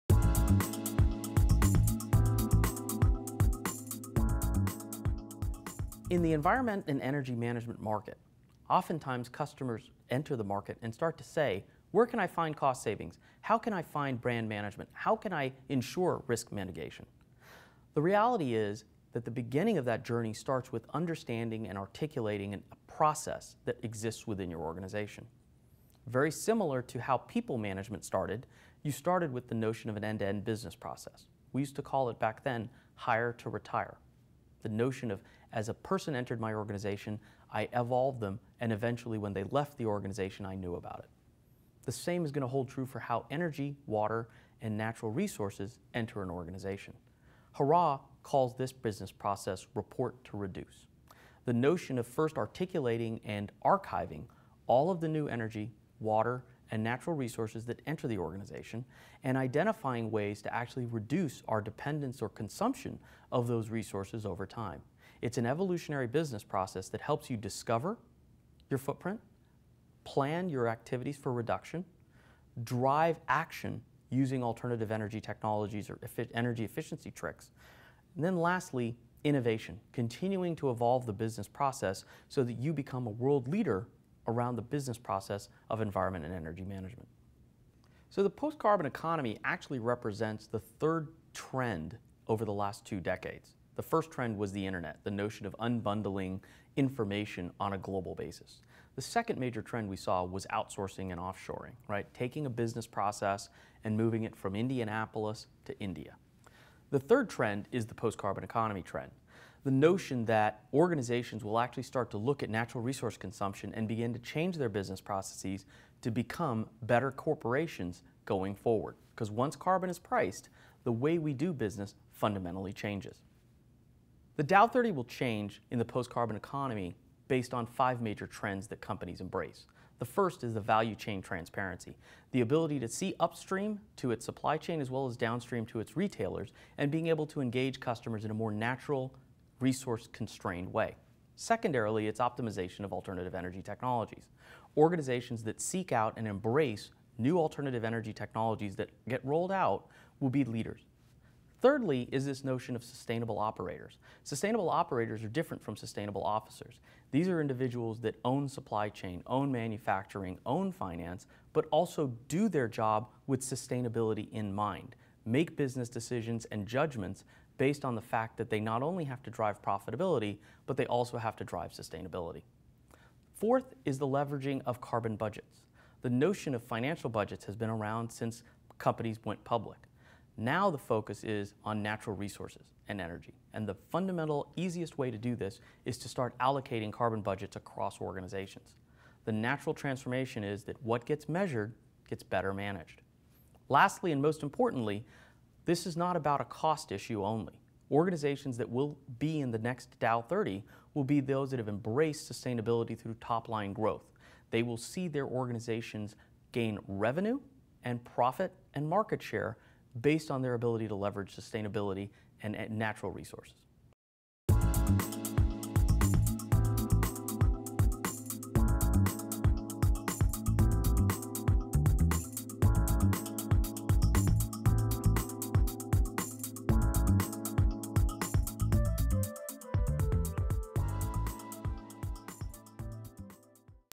Trends in Environmental and Energy Management
Hara CEO - Amit Chatterjee - discusses trends in the environmental and energy management space.